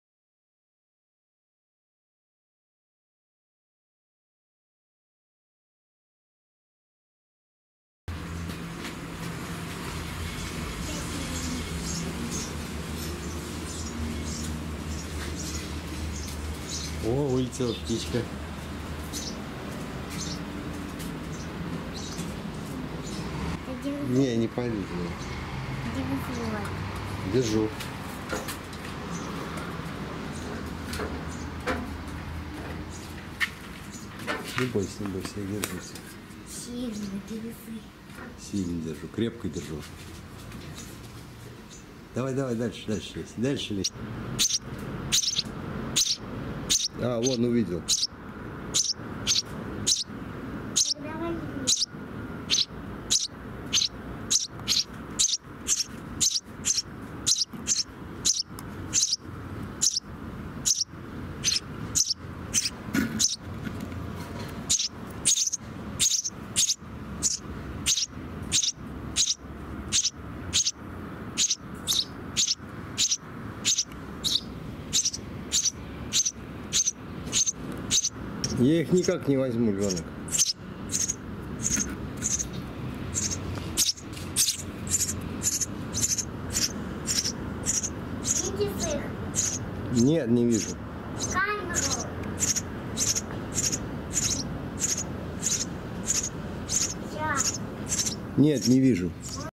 Гнездо воробьев
2016/07/01 уже оперившиеся птенцы, двоих (выпавших из гнезда или учившихся летать) съела кошка. Сколько их там осталось неизвестно.